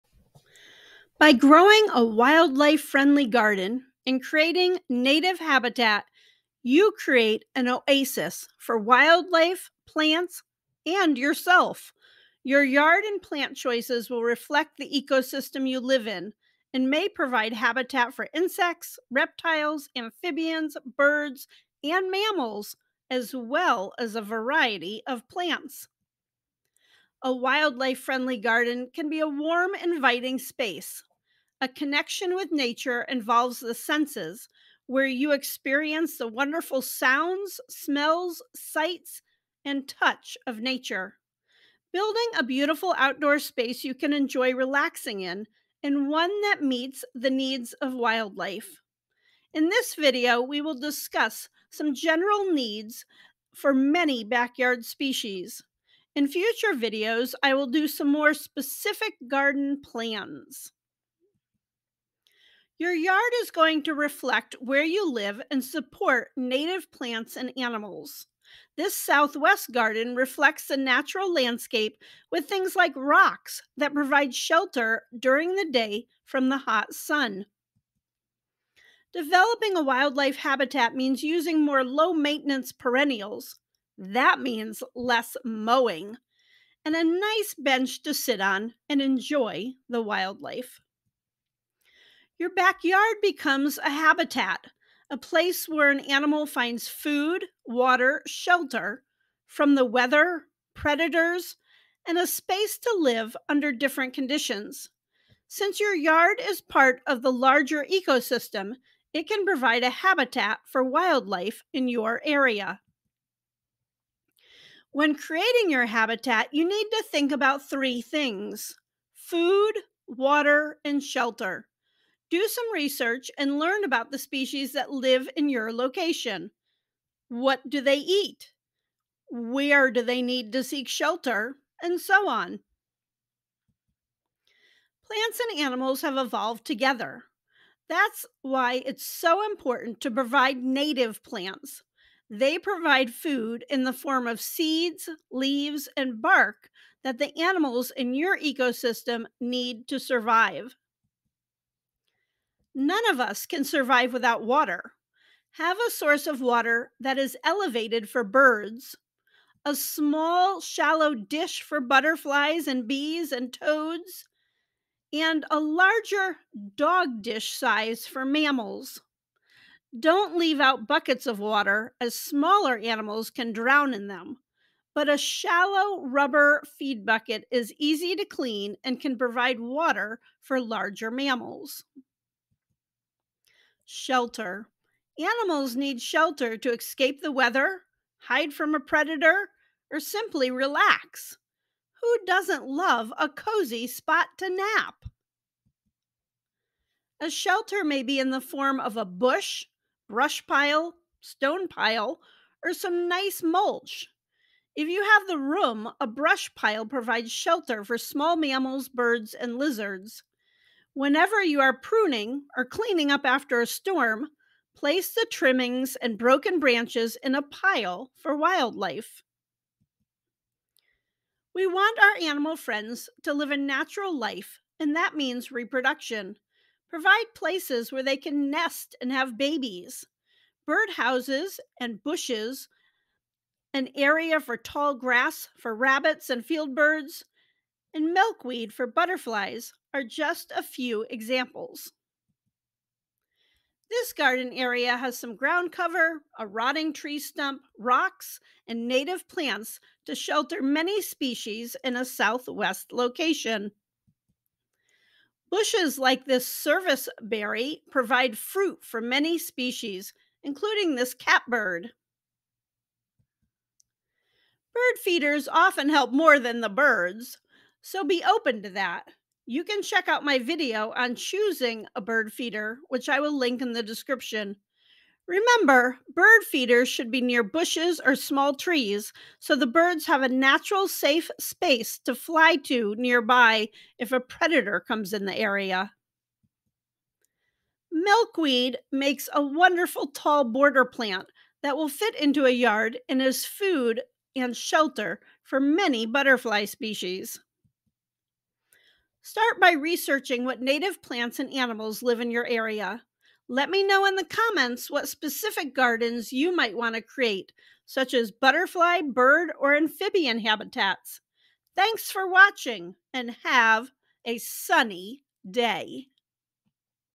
Create A Wildlife Friendly Garden
You can create a wildlife-friendly garden by providing three things. Food, Water, and Shelter.
Getting Started in Wildlife Rehabilitation
Diseases and Parasites in Wildlife Rehabilitation
Wildlife Tracks Activity Book For Kids